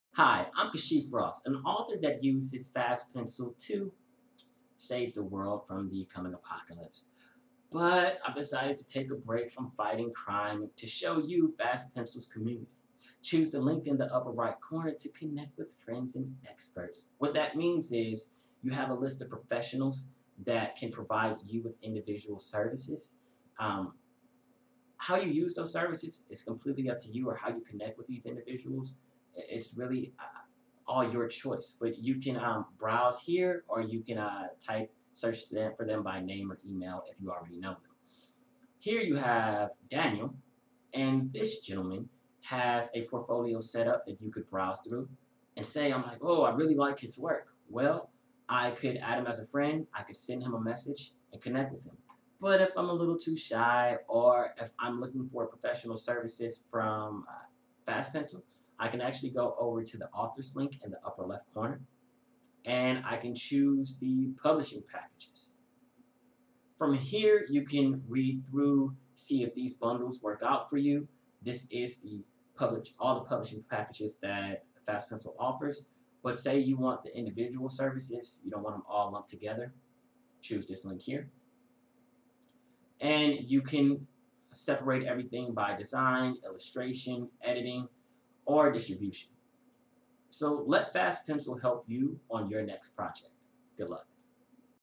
Finding Author Services Through the FastPencil Community
FastPencil super user Kashif Ross show you how to navigate to the FastPencil Community page so you can explore Author Services.